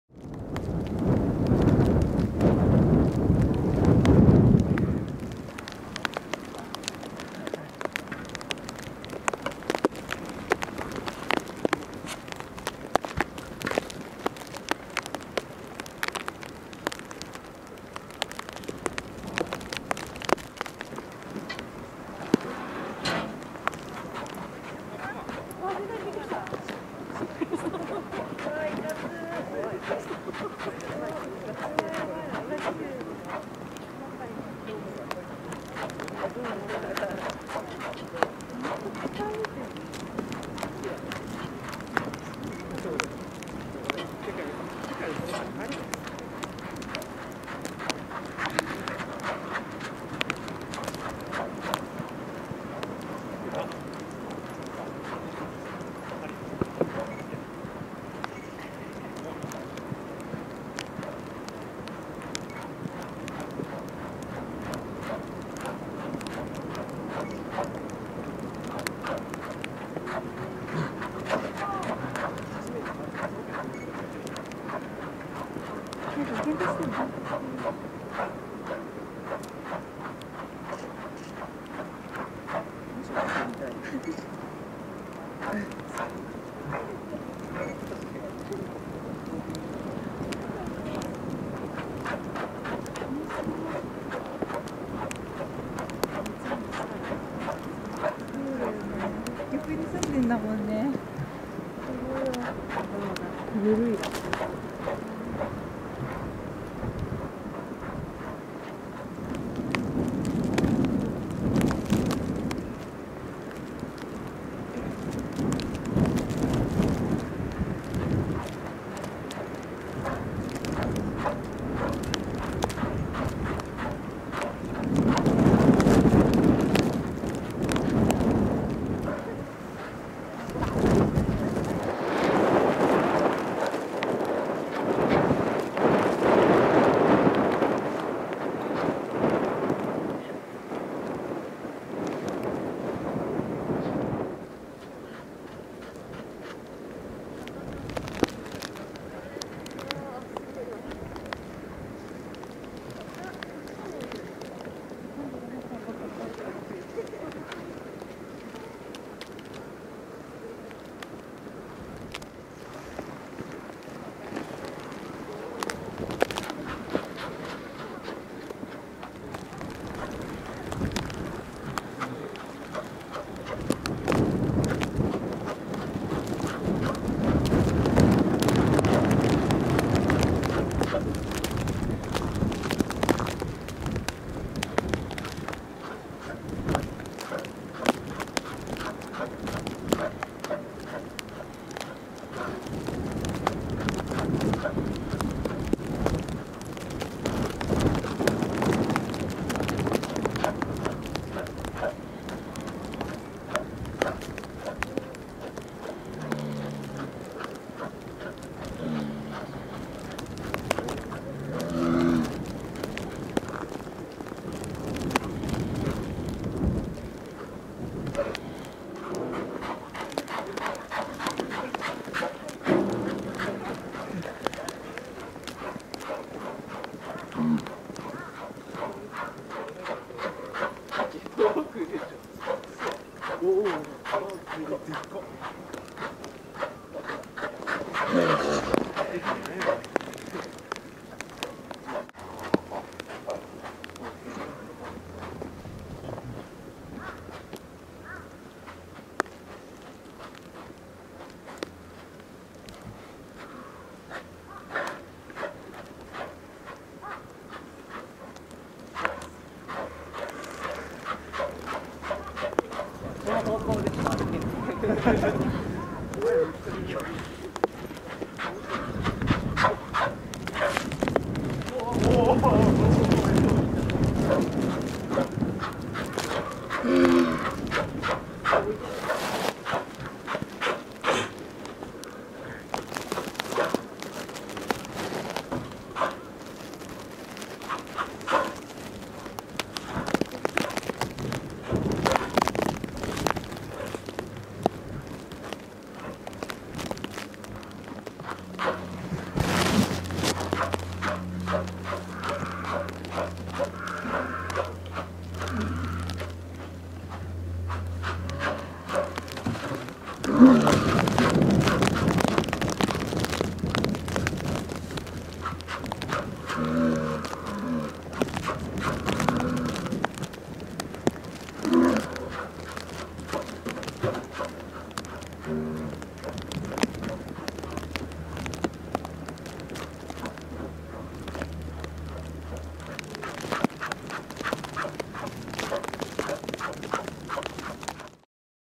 ララとデナリの同居開始 ララにプレゼントしたデナリ~Polar Bears
円山動物園のホッキョクグマ、ララ(メス/19歳)、キャンディ(メス/21歳)、デナリ(オス/20歳）。
今日は暴風雪。
ララとデナリの、挨拶程度の同居が行われました。
時間にして1時間くらいの同居でした。
ララの発情はまだのようで、デナリが近づくと嫌がっている模様。
デナリは、傍にあったポリタンクを、ララに差し出していました。
(2014年3月6日(木)　北海道札幌市　円山動物園にて撮影)